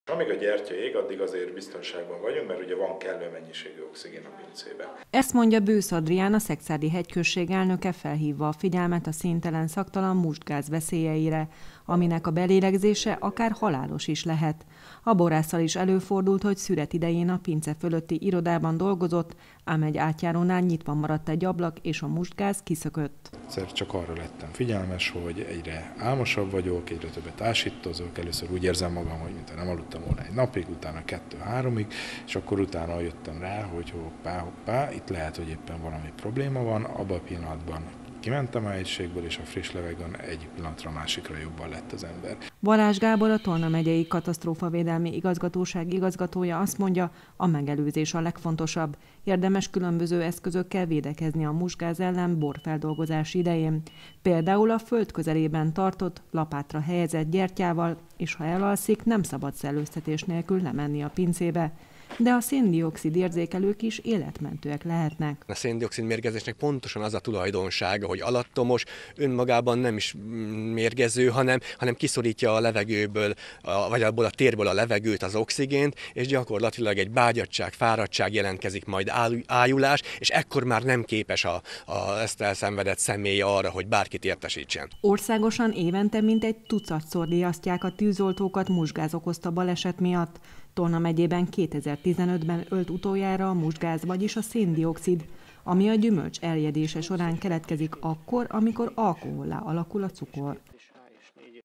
Mustgáz veszély 20190911
Országosan évente mintegy tucatszor riasztják a tűzoltókat mustgáz okozta baleset miatt. Tolna megyében 2015-ben ölt utoljára a mustgáz, vagyis a szén-dioxid. Pedig  szakemberek és borászok szerint egyszerű módszerekkel és egy kis odafigyeléssel megelőzhetőek a mustgázzal kapcsolatos pince balesetek. Fontos, hogy a borospincét üzemeltetők gondoskodjanak a pincék szellőztetéséről és tudatosan figyeljenek a jelekre.